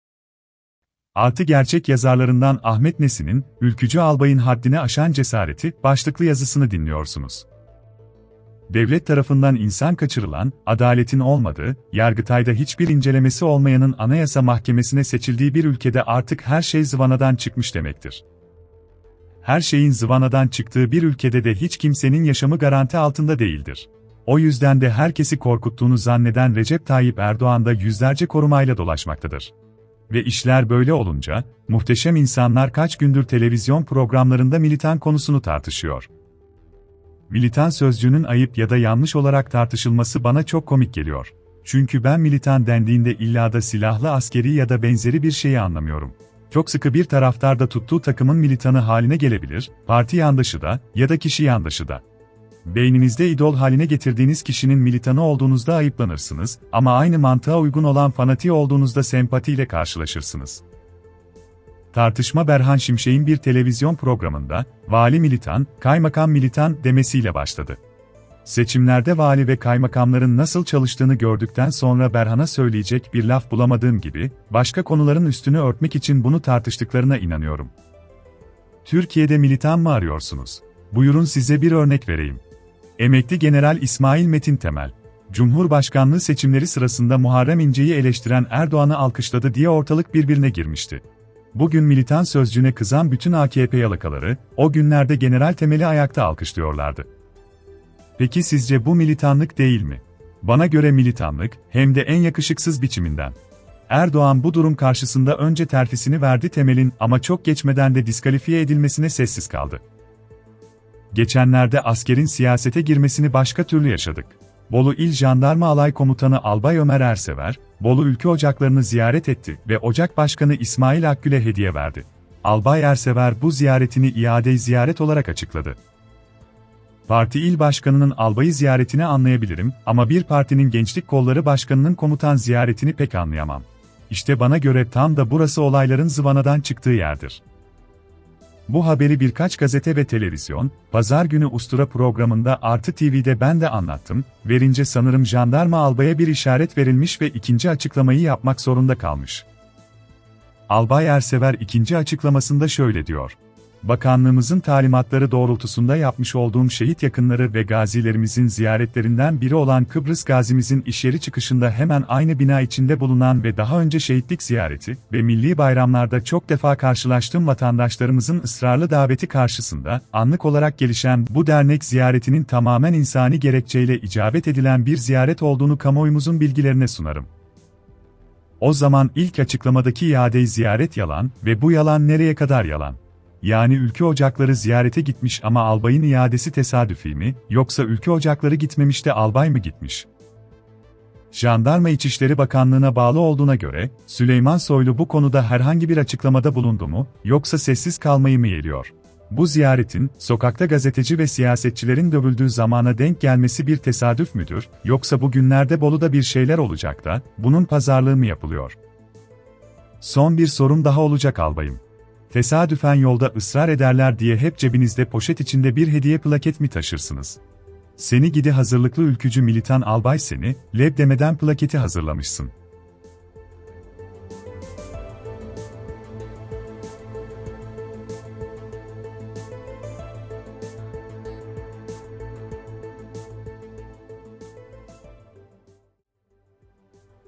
Ahmet Nesin - Ülkücü albayın haddini aşan cesareti...
Geçenlerde askerin siyasete girmesini başka türlü yaşadık. Bolu İl Jandarma Alay Komutanı Albay Ömer Ersever, Bolu Ülkü Ocakları'nı ziyaret etti ve ocak başkanı İsmail Akgül’e hediye verdi.